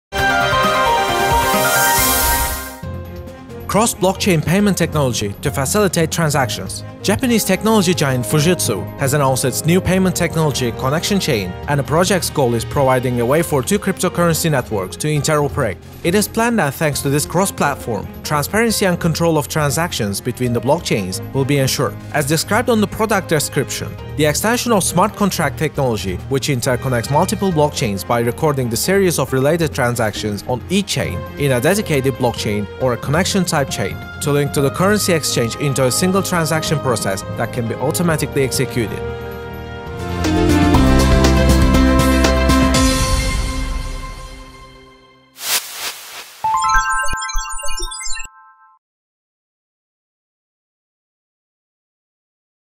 New payment technology based on blockchain
Cross-blockchain payment technology to facilitate transactions. Japanese technology giant Fujitsu, has announced its new payment technology ConnectionChain and the project’s goal is providing a way for two cryptocurrency networks to interoperate. It is planned that thanks to this cross-platform, transparency and control of transactions between the blockchains will be ensured. As described on the product description; ‘’The extension of smart contract technology which inter-connects multiple blockchains by recording the series of related transactions on each chain in a dedicated blockchain, or a "connection-type chain," to link to the currency exchange into a single transaction process that can be automatically executed.’’